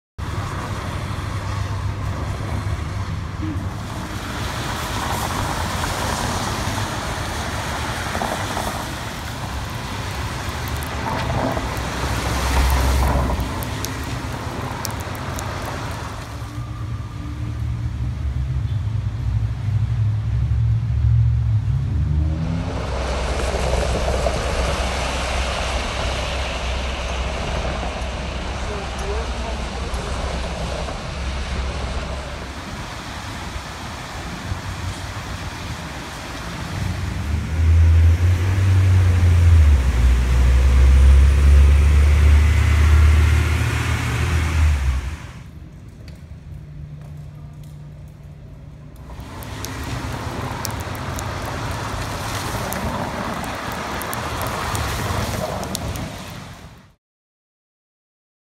News Video - Rain Comes to Bakersfield
Californian photographer Henry Barrios captured some video of today's rain in Bakersfield.